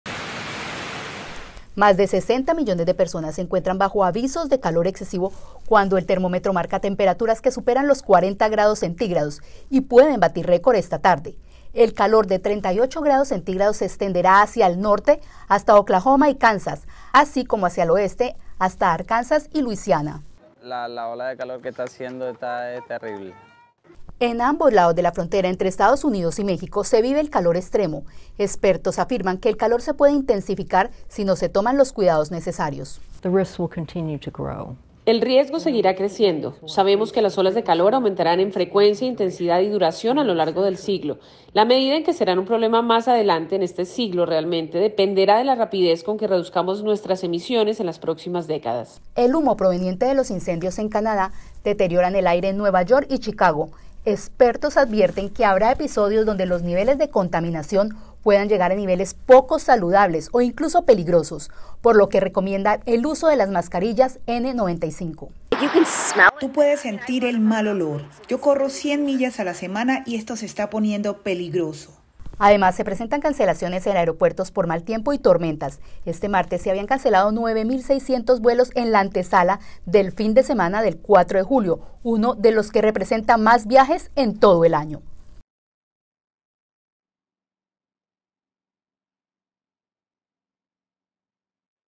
EUA enfrenta intensa ola de calor, mala calidad de aire y cancelación de vuelos
Las autoridades emitieron un alerta por calor excesivo desde Arizona hasta Florida, mientras los incendios de Canadá generan mala calidad de aire en varias ciudades del norte del país.